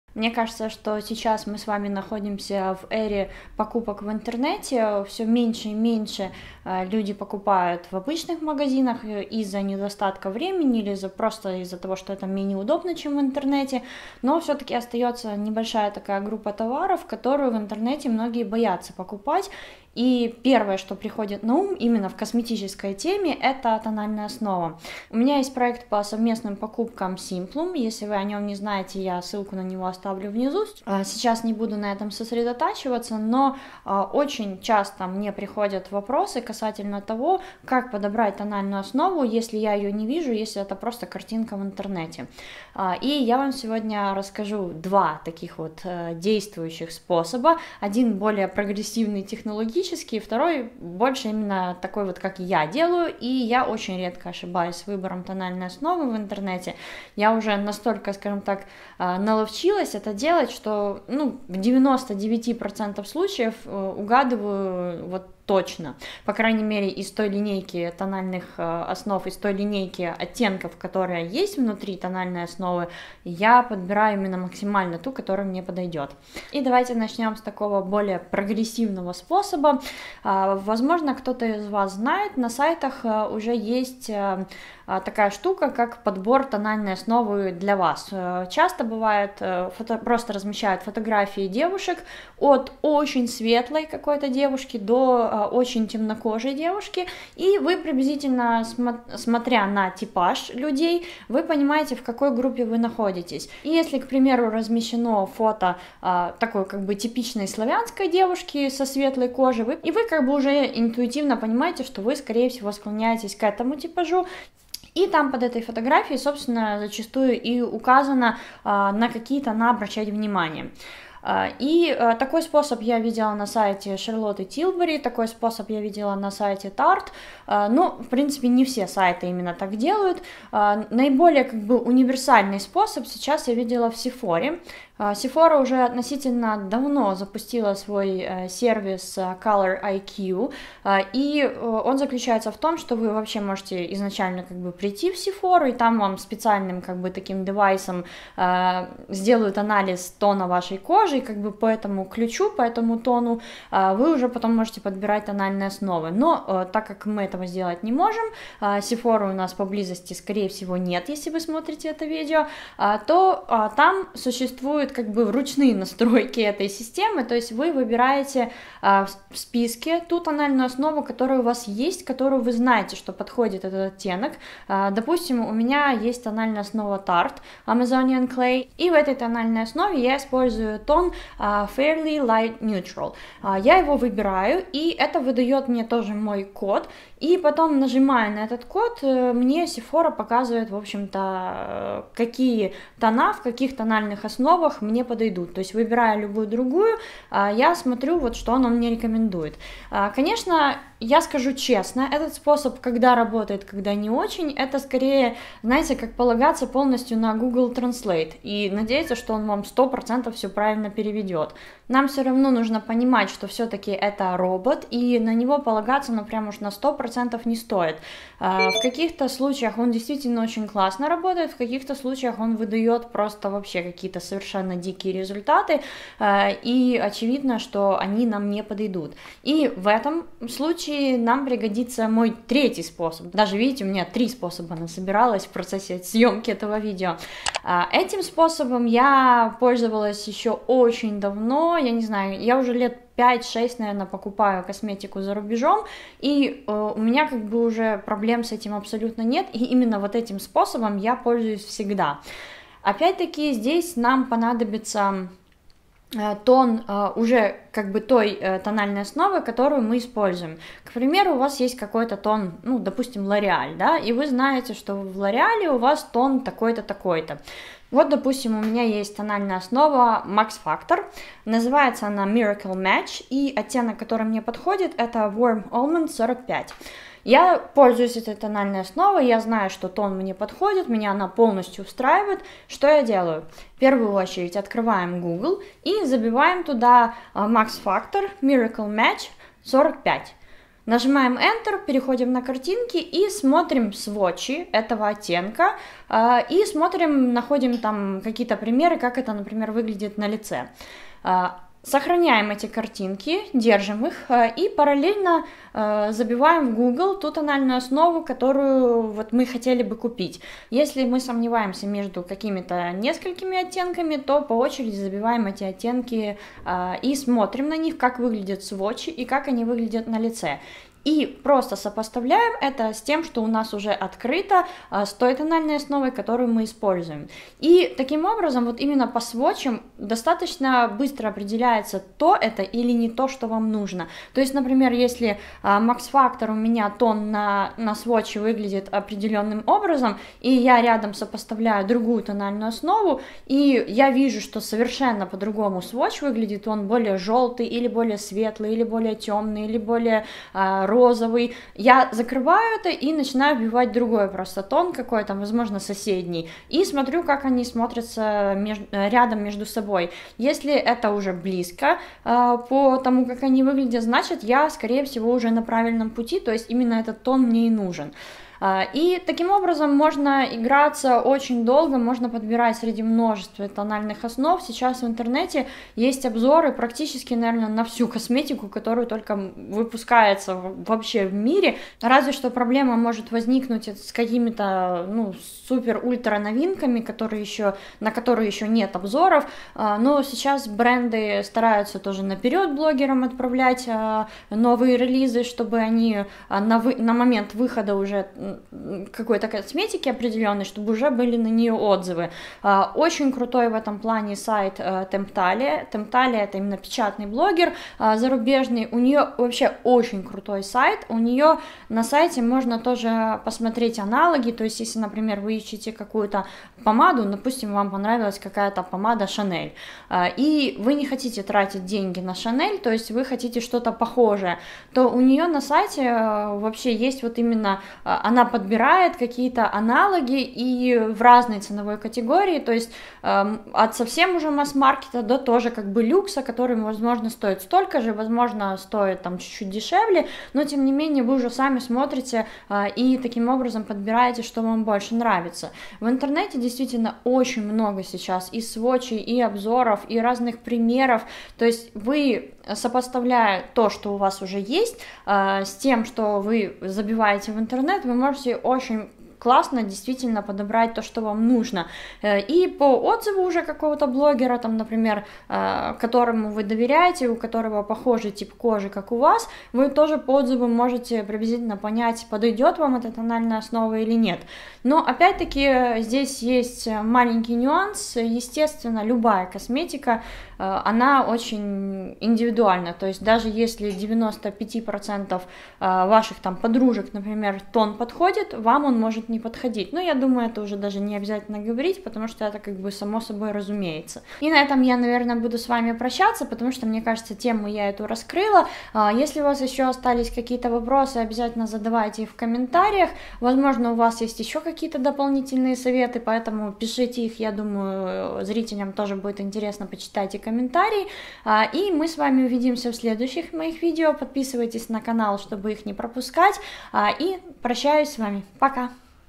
Как подобрать тональный крем в интернете | Советы визажиста
Как не ошибиться с тоном? как выбрать правильную тональную основу? На что обращать внимание? Где заказать? Обо всем этом рассказываю в видео!